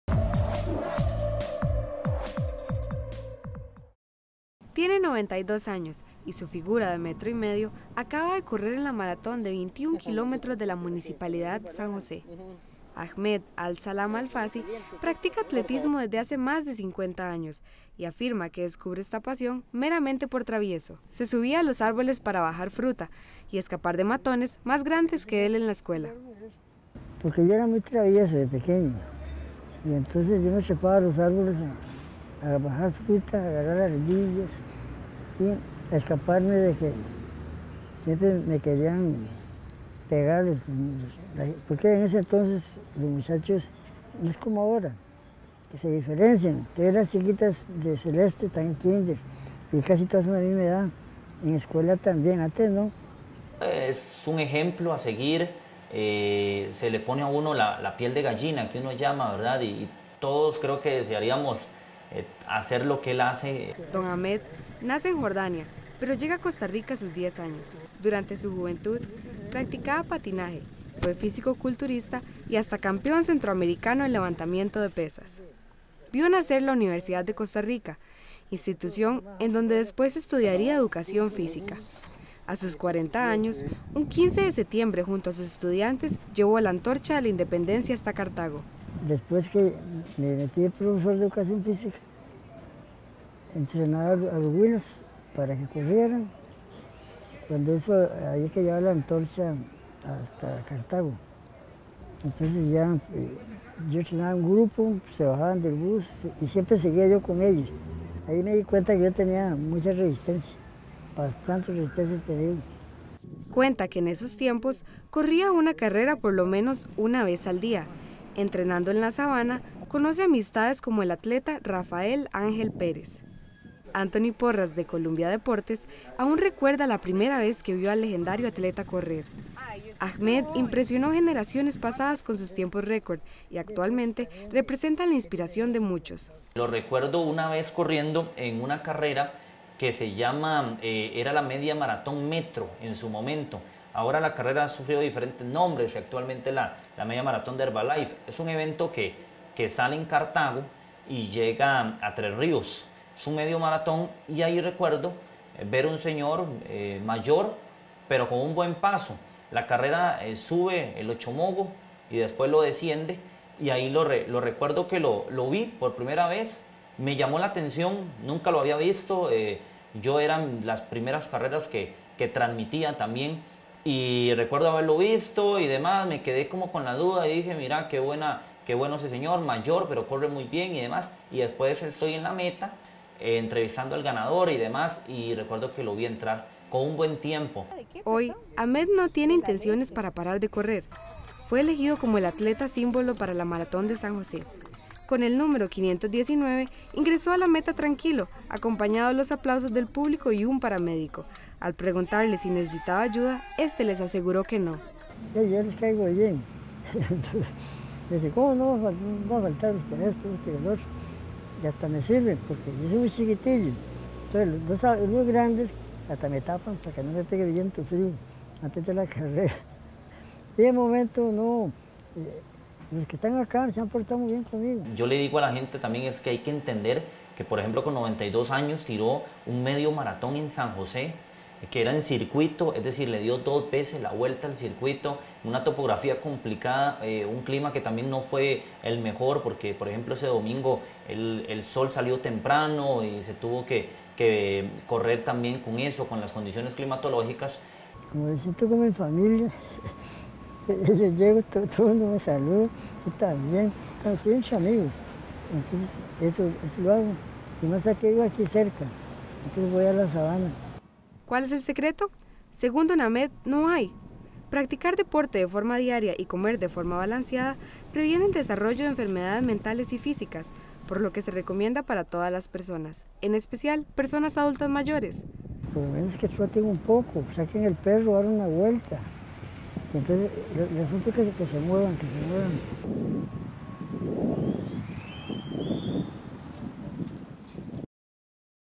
Personaje de Oro: Ahmed
Ahmed, es una persona que corre maratones de 21 kilómetros. Conozca a este atleta de 99 años en nuestra sección Personaje de Oro.